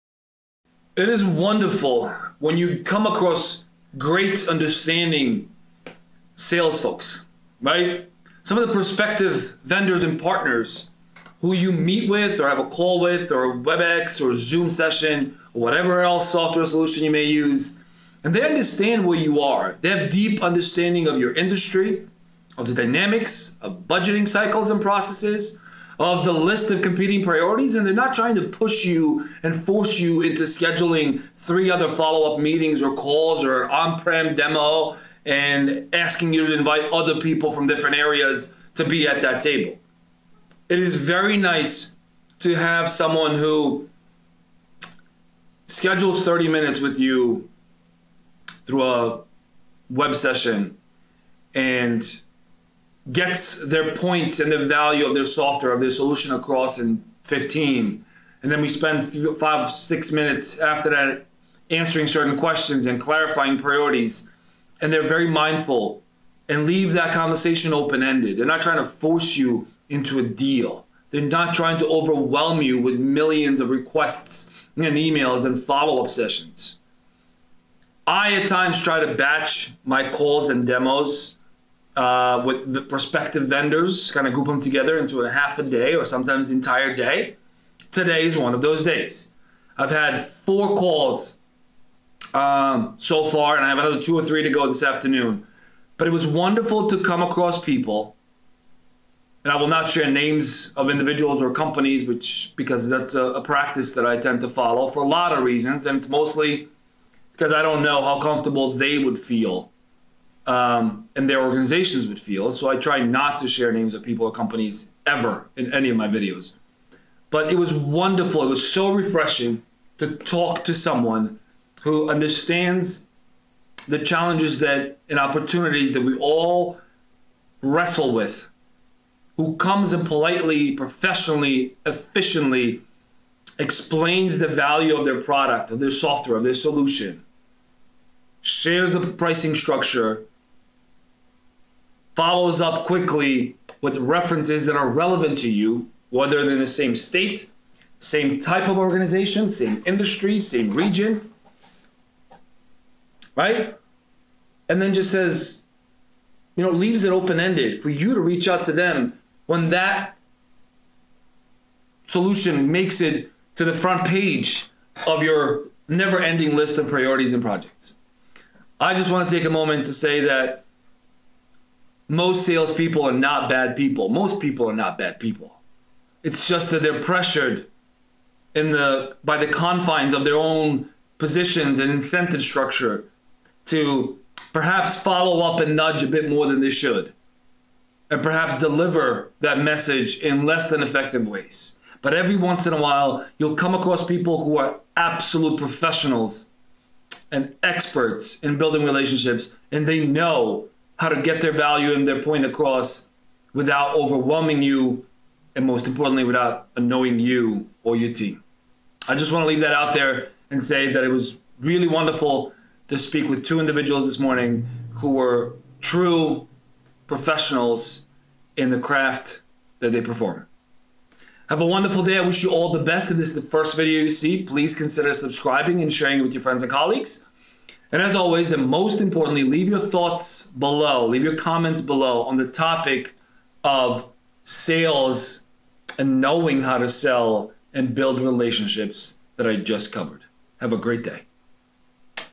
Working with professional sales people
It is a true pleasure when you speak with people who are aware of your challenges and competing priorities, who have deep understanding of your industry and who do not try to pressure you or your team into further engagements via additional calls, meetings or demos.

Those true professionals demonstrate the value of their product or a service, share pricing information and make introductions to relevant references. Then, they provide time and space for us internally to discuss, evaluate and reach out to them at our pace.